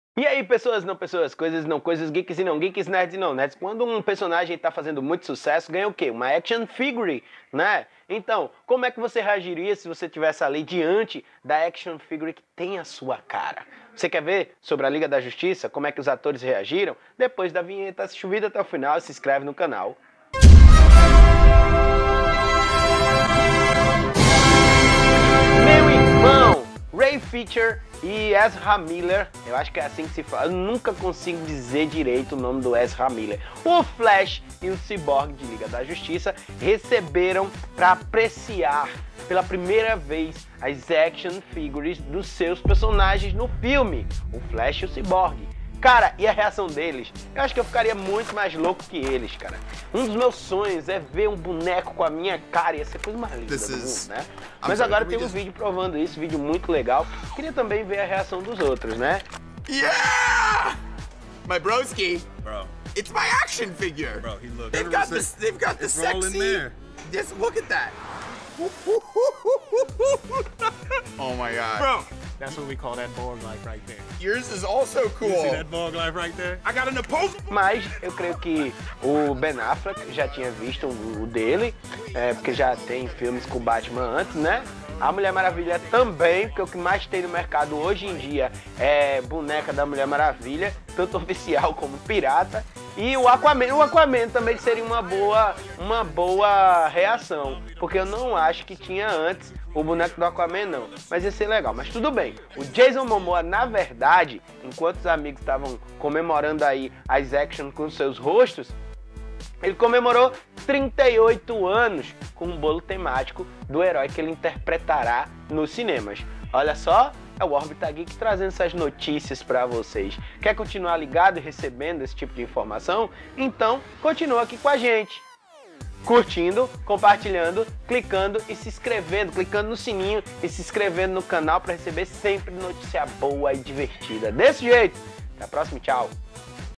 Liga da Justiça reação com Bonecos e Niver de Jason Momoa
O filme da Liga da Justiça está chegando e os bonecos dos Heróis já estão a venda, os atores receberam os seus e veja a reação ao se ver. E o aquaman comemorou seu aniversário !